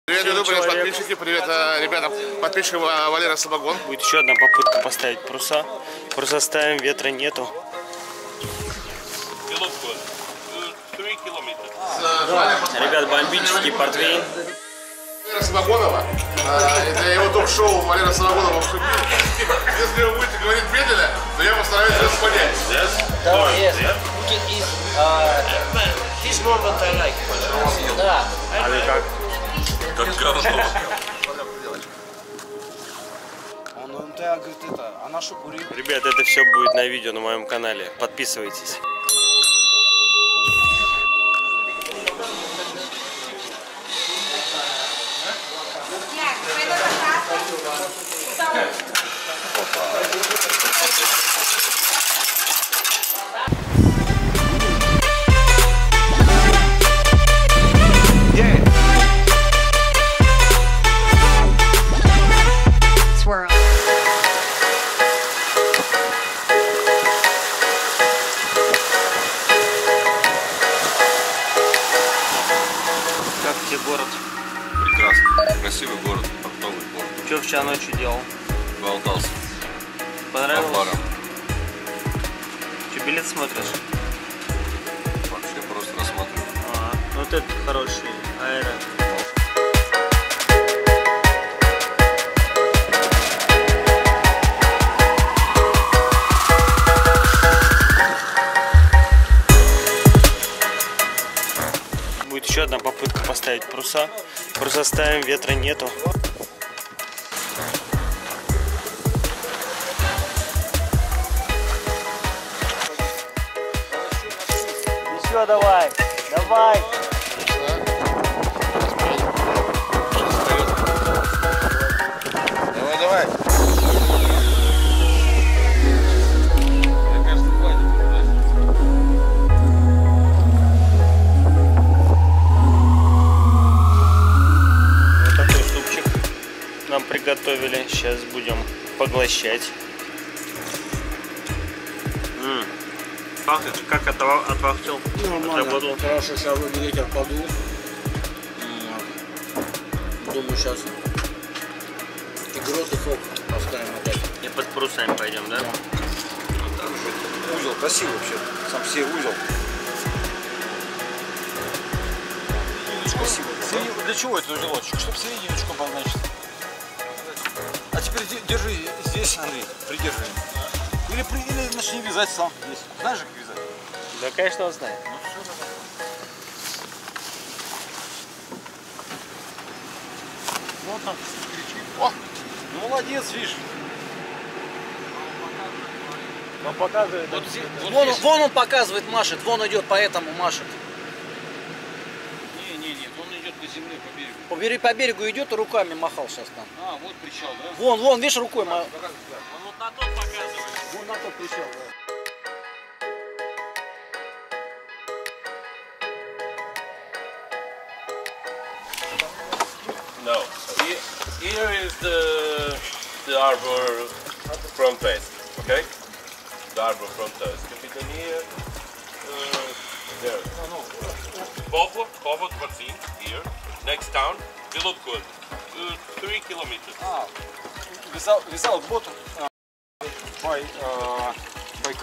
Приехали в пригород города Порто
Кругосветка продолжается новая серия на канале!

Кругосветное путешествие с друзьями на поморской ладье Пилигрим (Pilgrim)

Мой Инстаграм @valera_samogon